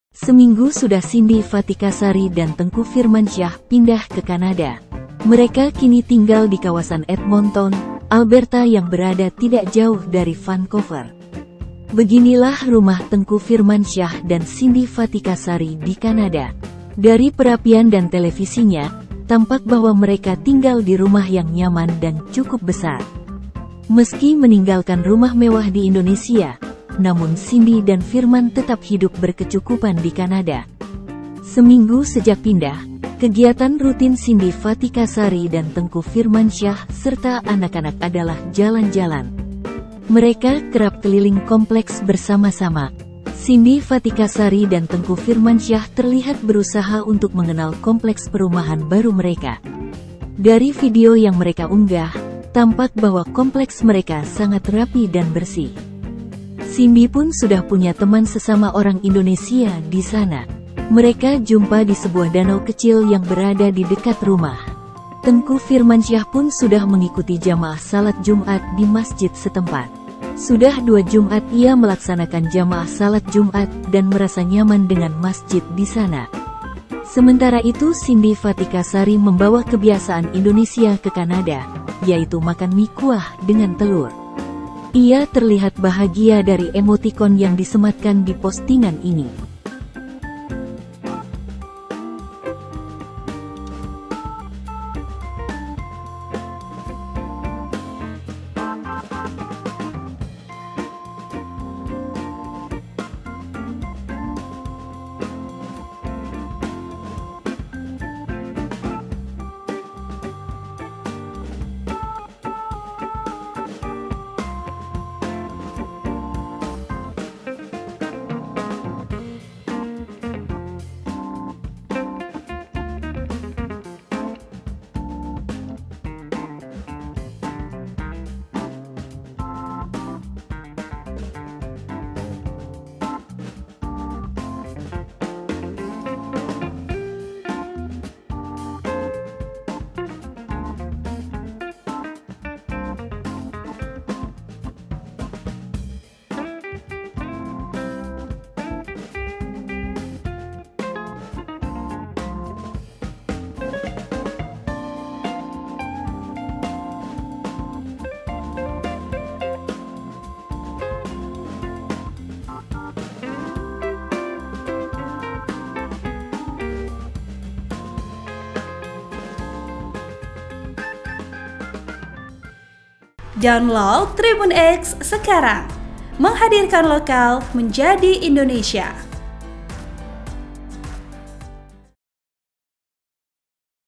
Potret Rumah Baru Cindy Fatikasari dan Tengku Firmansyah di Kanada, Lingkungan Rapi!
Seminggu sudah Cindy Fatikasari dan Tengku Firmansyah pindah ke Kanada. Mereka kini tinggal di kawasan Edmonton, Alberta yang berada tidak jauh dari Vancouver.

Beginilah rumah Tengku Firmansyah dan Cindy Fatikasari di Kanada. Dari perapian dan televisinya, tampak bahwa mereka tinggal di rumah yang nyaman dan cukup besar.

Meski meninggalkan rumah mewah di Indonesia, namun Cindy dan Firman tetap hidup berkecukupan di Kanada.

Seminggu sejak pindah, kegiatan rutin Cindy Fatikasari dan Tengku Firmansyah serta anak-anak adalah jalan-jalan. Mereka kerap keliling kompleks bersama-sama.

Cindy Fatikasari dan Tengku Firmansyah terlihat berusaha untuk mengenal kompleks perumahan baru mereka.

Dari video yang mereka unggah, tampak bahwa kompleks mereka sangat rapi dan bersih.

Cindy pun sudah punya teman sesama orang Indonesia di sana. Mereka jumpa di sebuah danau kecil yang berada di dekat rumah.

Tengku Firmansyah pun sudah mengikuti jamaah salat Jumat di Masjid setempat. Sudah 2 Jumat ia melaksanakan jamaah salat Jumat dan merasa nyaman dengan Masjid di sana.

Sementara itu Cindy Fatikasari membawa kebiasaan Indonesia ke Kanada, yaitu makan mie kuah dengan telur.

Ia terlihat bahagia dari emoticon yang disematkan di postingan ini.